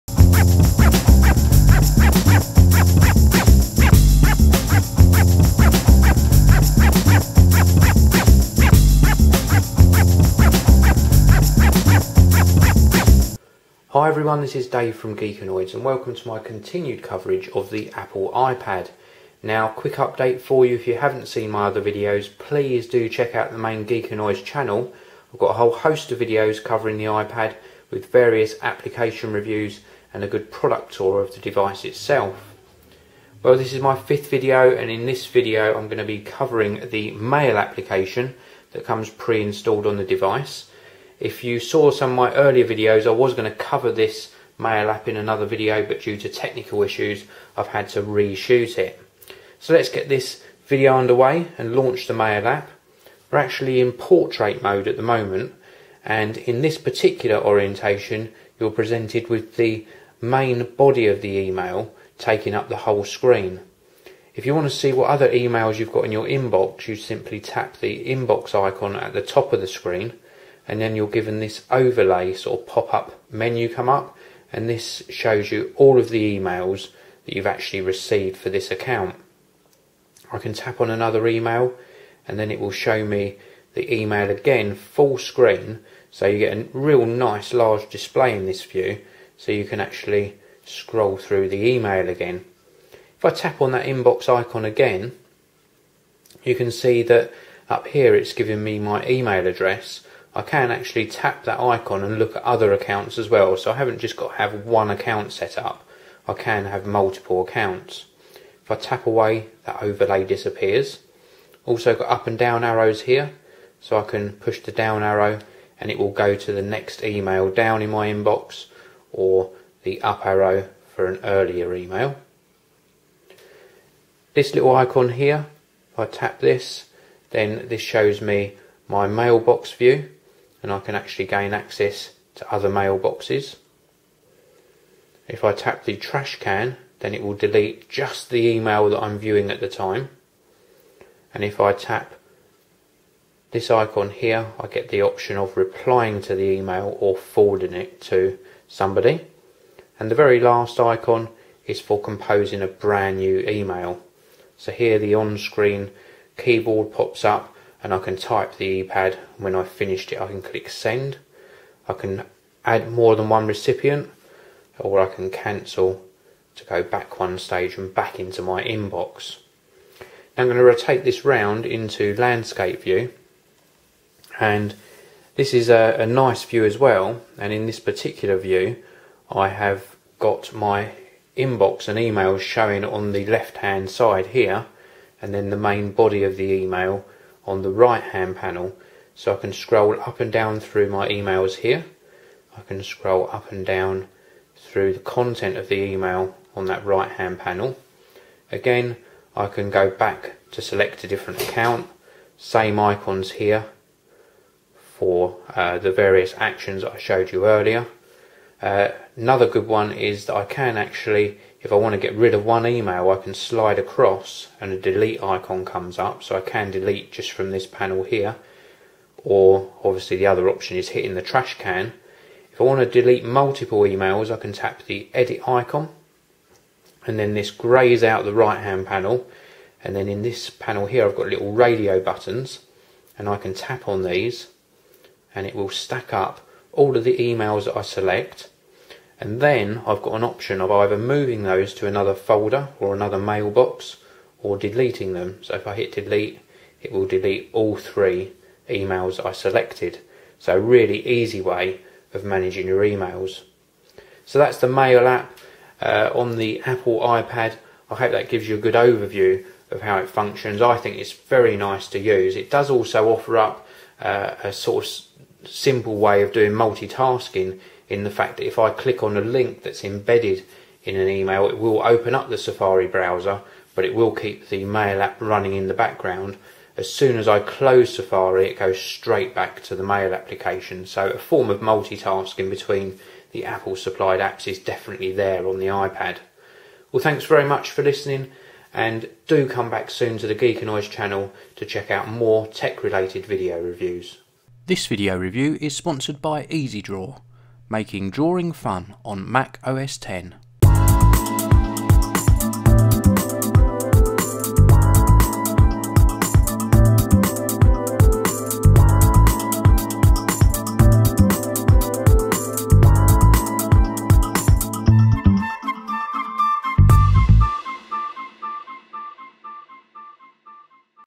Apple iPad (64GB) - Part 5 - Apple Apps - Mail
Apple iPad (64GB) - Part 5 - Apple Apps - Mail. In this video I show you how the Mail application works on the iPad.